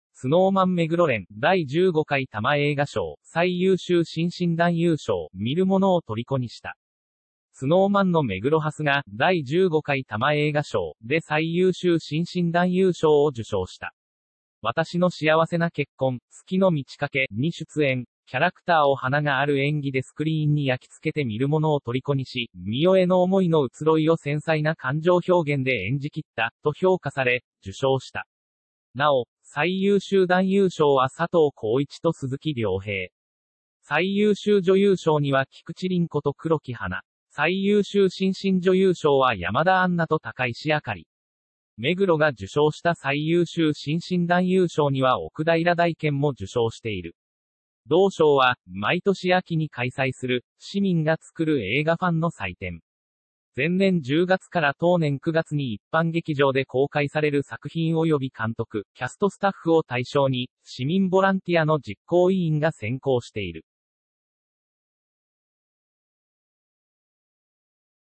Snow Man目黒蓮「第15回TAMA映画賞」最優秀新進男優賞「観る者をとりこにした」
Snow Manの目黒蓮が『第15回TAMA映画賞』で最優秀新進男優賞を受賞した。 　『わたしの幸せな結婚』『月の満ち欠け』に出演。「キャラクターを華がある演技でスクリーンに焼き付けて観る者をとりこにし、美世への想いの移ろいを繊細な感情表現で演じきった」と評価され、受賞した。 　なお、最優秀男優賞は佐藤浩市と鈴木亮平。最優秀女優賞には菊地凛子と黒木華。最優秀新進女優賞は山田杏奈と髙石あかり。目黒が受賞した最優秀新進男優賞には奥平大兼も受賞している。 　同賞は、毎年秋に開催する、市民がつくる映画ファンの祭典。前年10月から当年9月に一般劇場で公開される作品及び監督・キャスト・スタッフを対象に、市民ボランティアの実行委員が選考している。

IMPORTANT!